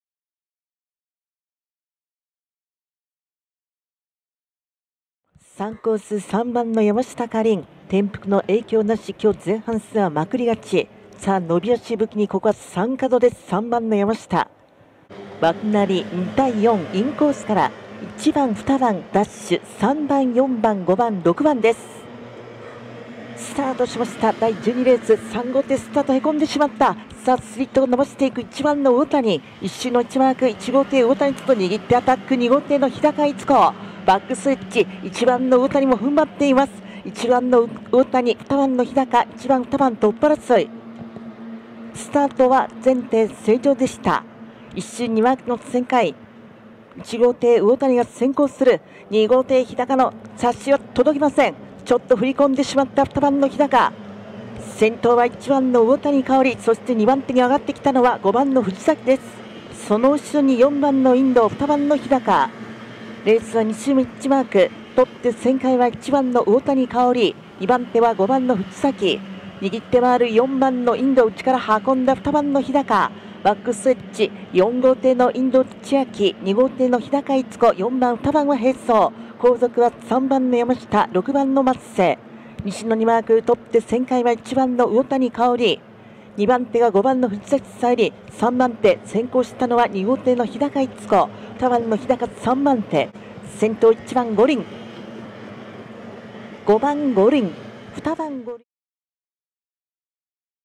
遊んでる？３カドから突然進路変更③山下夏鈴【競艇・珍進入】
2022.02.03 若松公営レーシングプレス杯　男女Ｗ優勝戦3日目12R